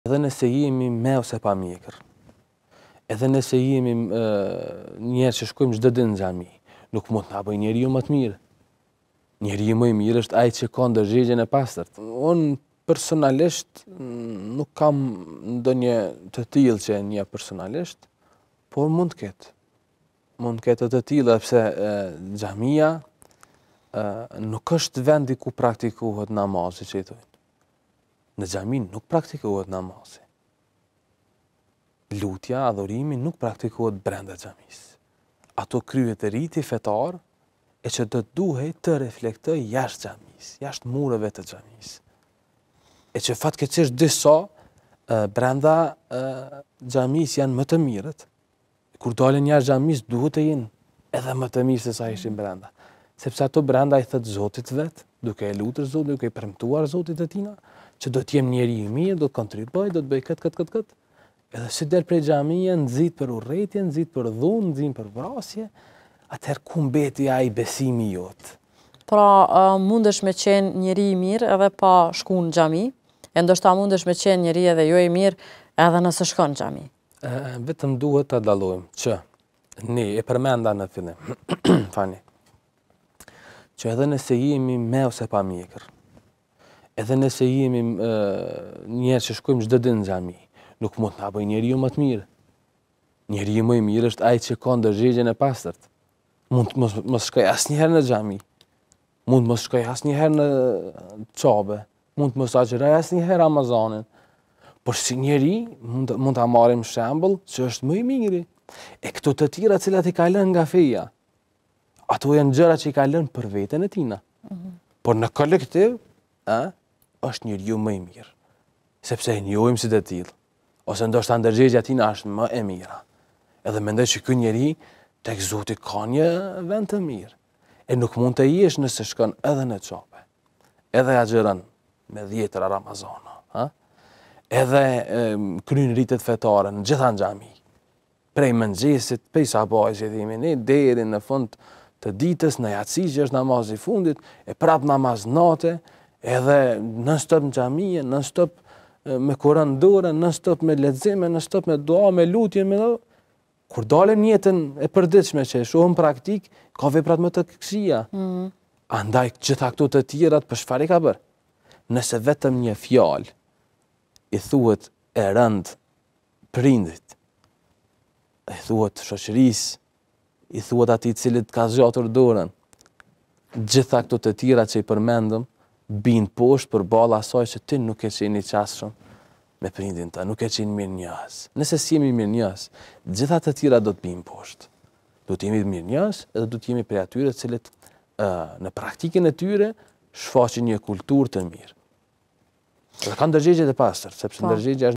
Hoxhë Kujtim Hyseni: Nëse shkon në xhami s’bëhesh njeri më i mirë
“Nëse shkon në xhami s’bëhesh njeri më i mirë / Hoxhë Kujtim Hyseni: Në xhami kryhet riti fetar, s’praktikohet namazi